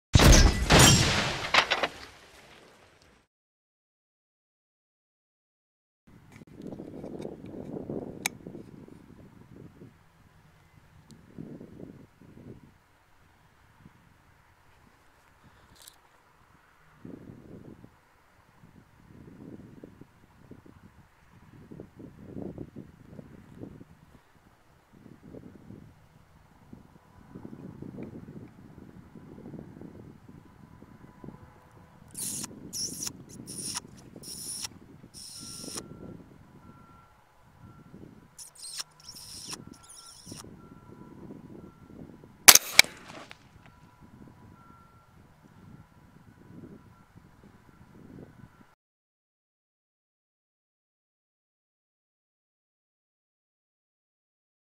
Night Coyote Hunt with ATN X-Sight II @ 200 yards
Check out this California night coyote hunt. Sometimes everything doesn't always fall in place how you want it, but anytime you can get a coyote between your cross-hairs on a coyote its always a good feeling. This shot was taken at 200 yards.